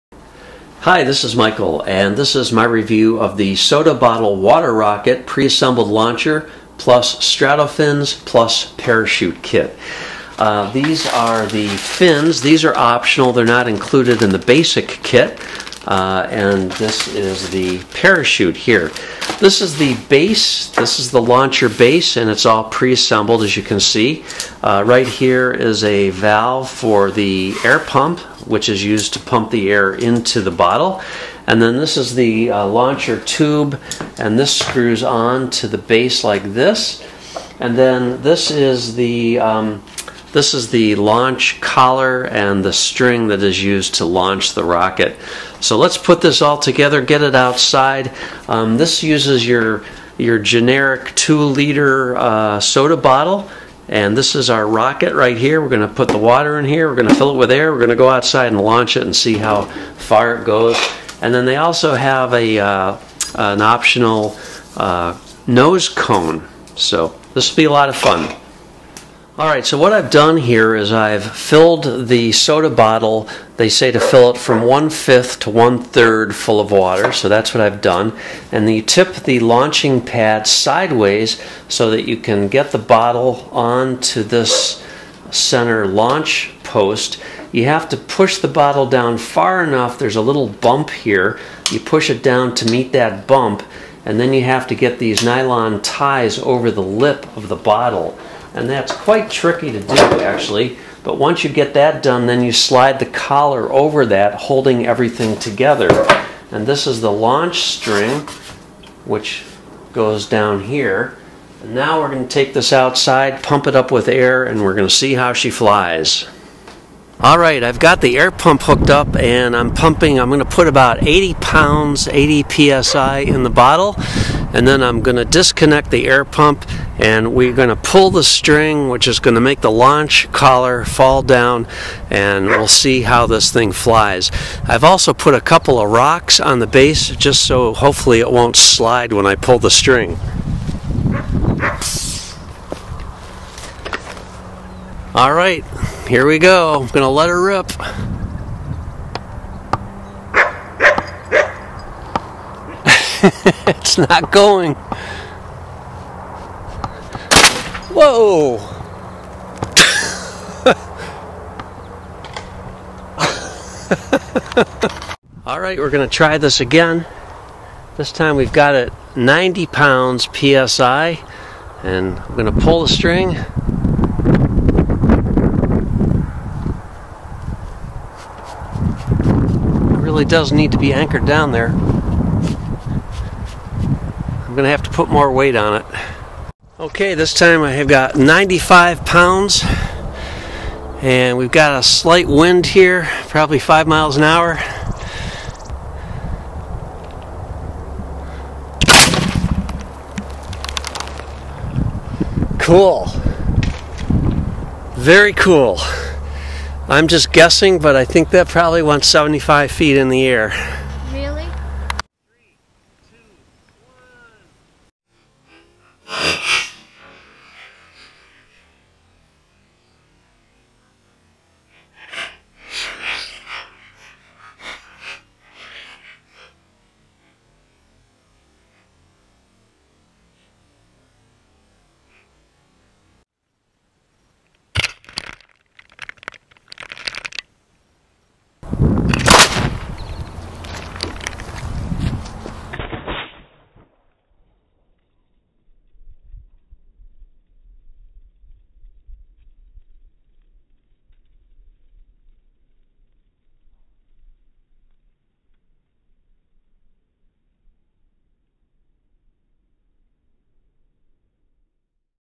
Review Soda Bottle Water Rocket Preassembled Launcher Toy
Totally fun but a little dangerous for use  unsupervised children due to the high pressure and consequently high velocity of the soda bottle during launch. It's a little finicky to get working right off the bat but once you have more experience with it, it begins to get much easier to launch.